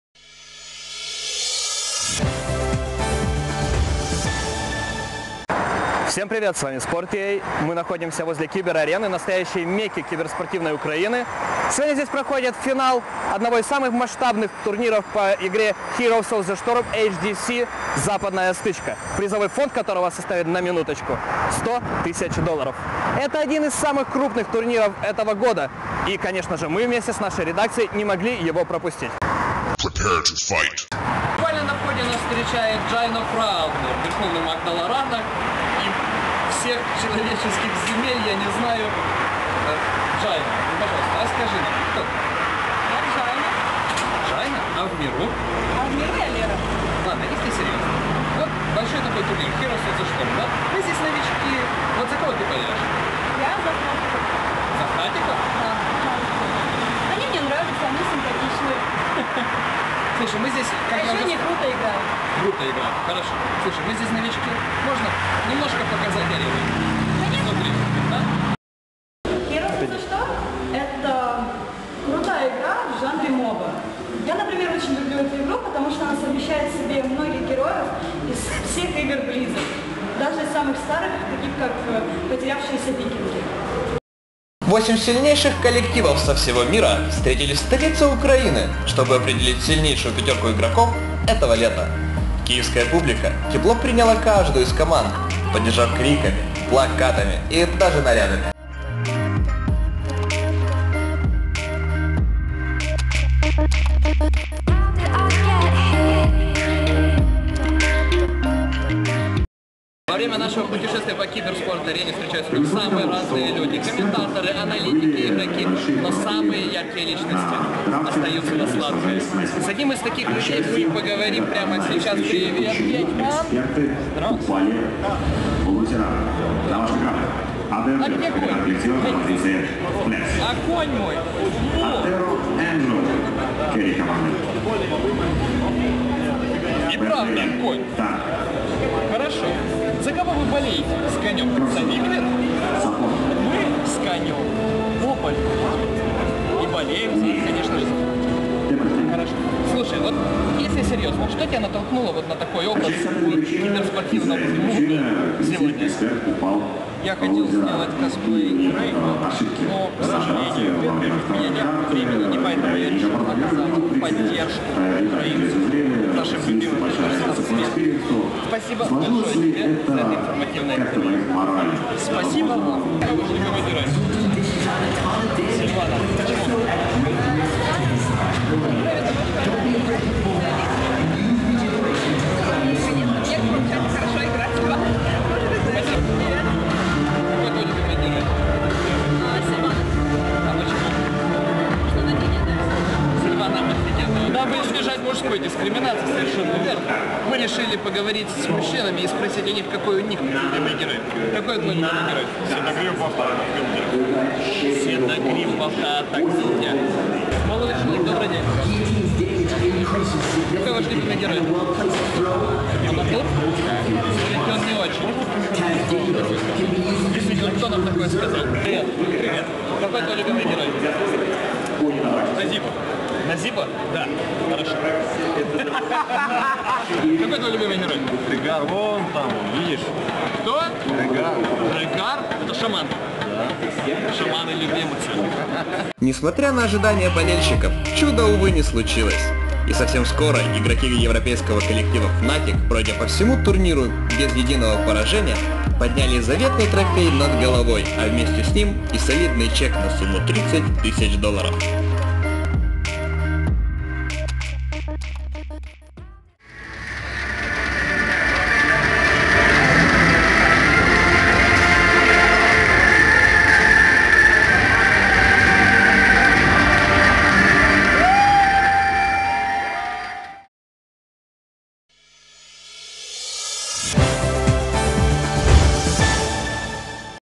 Общительный Репортаж с HGC - Западная Стычка
Репортеры Sport.ua посетили один из самых крупных турниров по Heroes of the Storm